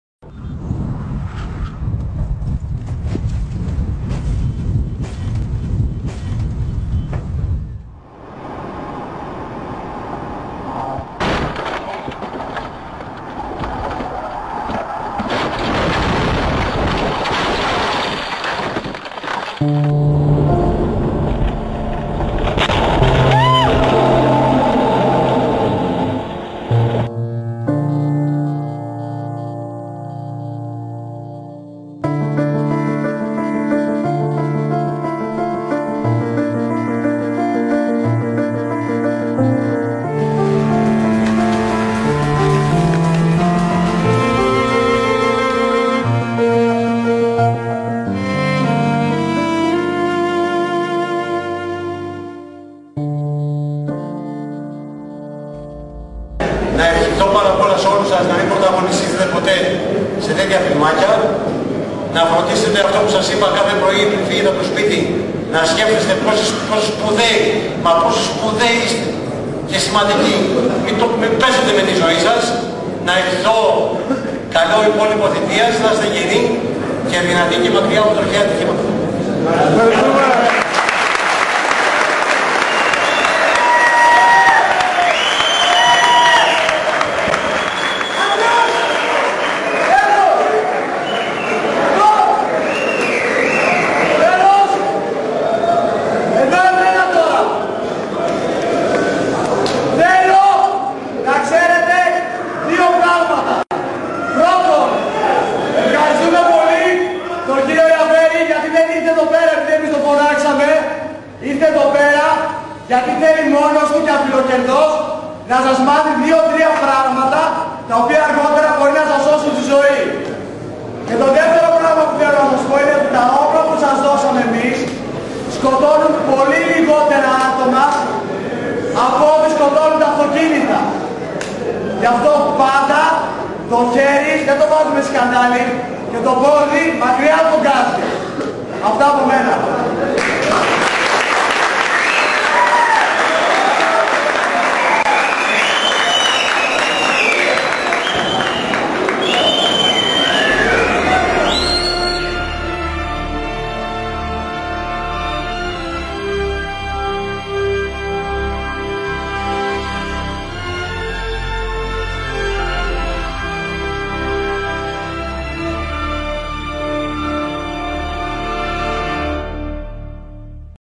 Το πόδι χαλαρά στο γκάζι | Ιαβέρης
Ένα από τα εκατοντάδες ΔΩΡΕΑΝ σεμινάρια σε στρατόπεδα, με αποδέκτες νέους στρατεύσιμους και αξιωματικούς!!!
Σιγά και προσεκτικά, πάντα με ζώνη και κράνος!!!